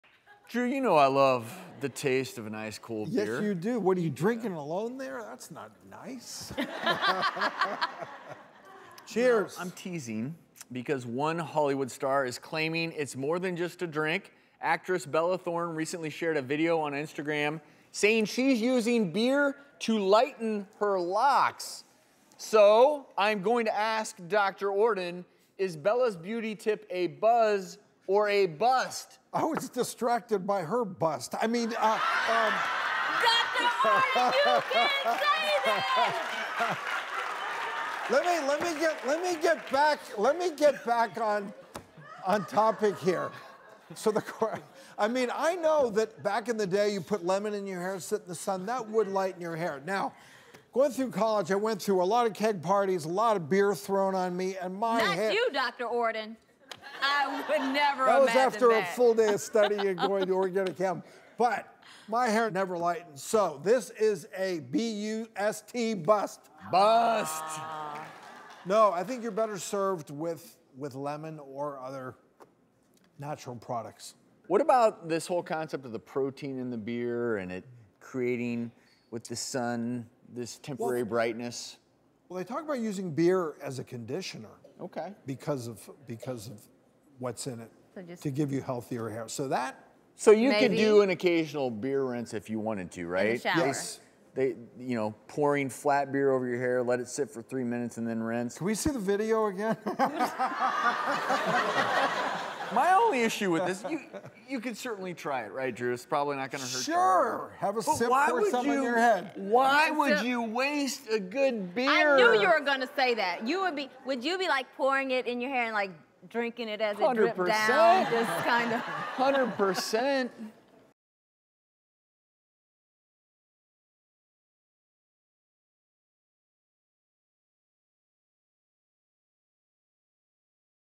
Bella Thorne Uses Beer to Lighten Her Hair – Does It Work?
Actress Bella Thorne shares that she uses beer to help lighten her hair. Dr. Andrew Ordon weighs in about whether or not this tip is a buzz, or a bust. Before you crack open a beer and take it to the shower with you, find out if there are any benefits to this trendy tip.